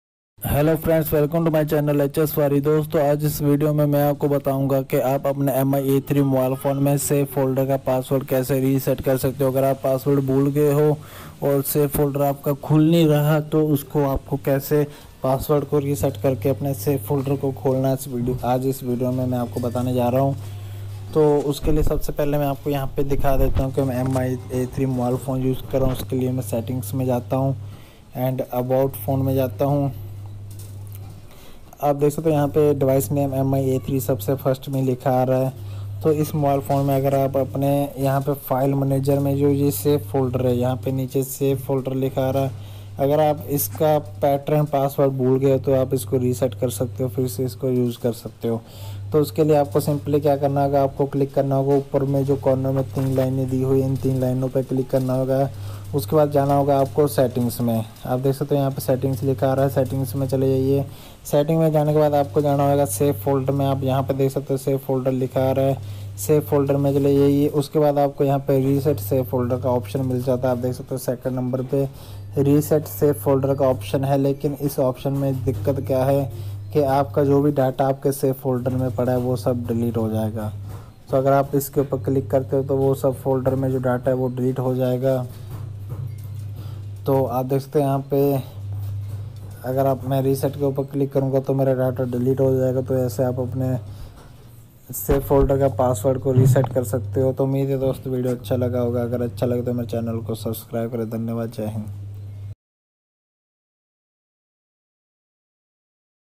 Mi A3 mobile phone mein safe folder ka password kaise reset Karen
doston is video mein main apni taraf se aap ko samjhane ke liye Puri koshish ki hai agar aapko fir bhi kuchh samajh nahin a Raha to aap mujhe comment karke likh sakte ho ho @HS Bari